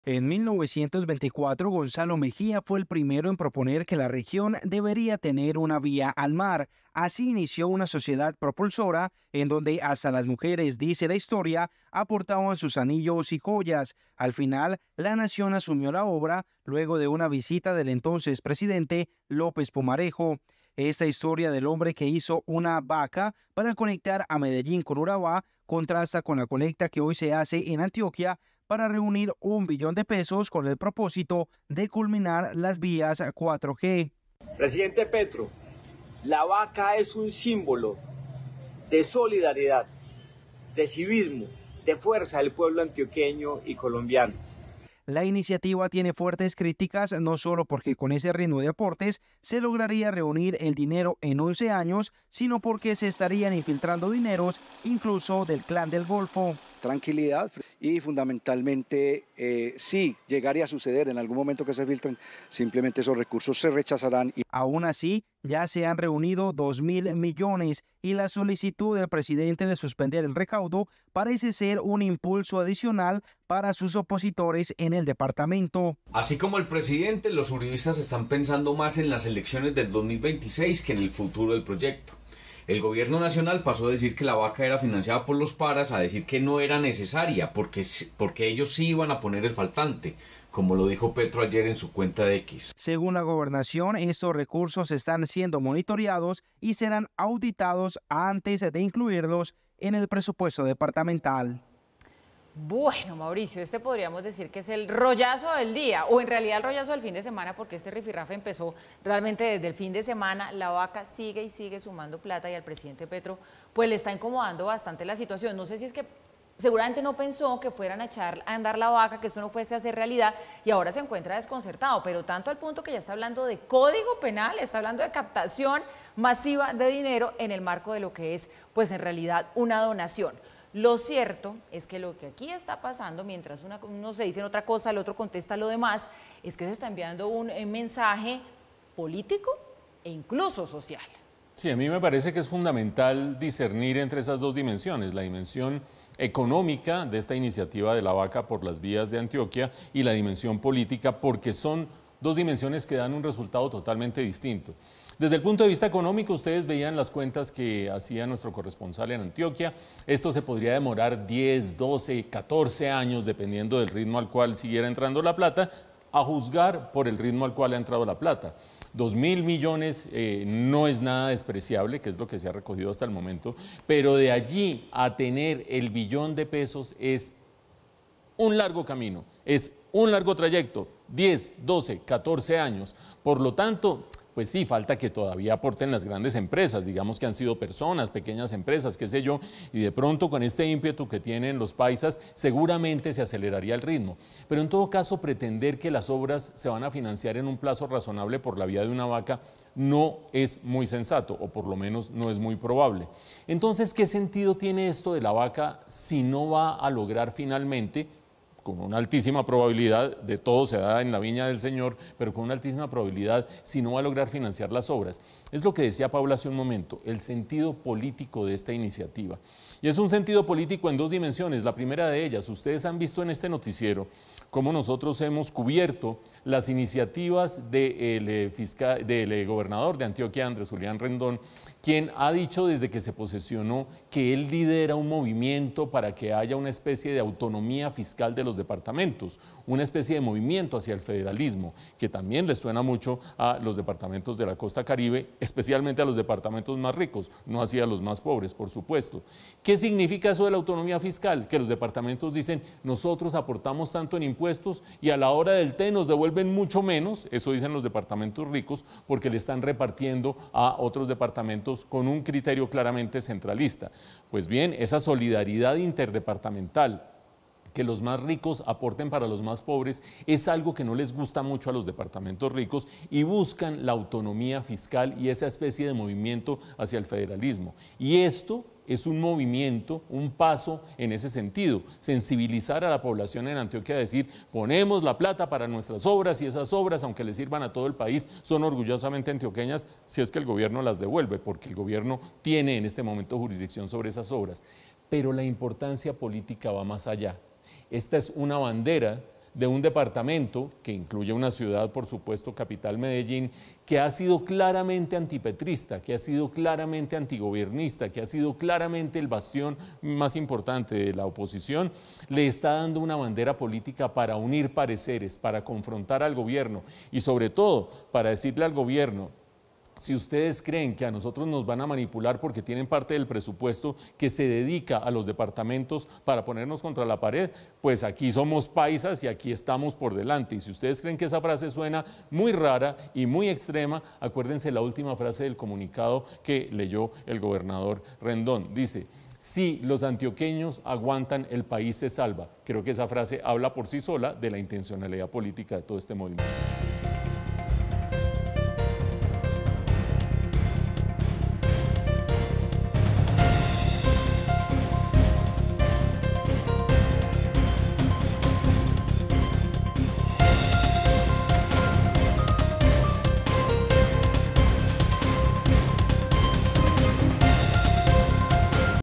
Antioquia sigue adelante con la 'vaca' para las 4G ¿reunirán el dinero necesario? | Red+
Opiniones de todo tipo han surgido sobre este tema ¿alcanzará Antioquia la meta? ¿Es sensato seguir buscando contribuciones?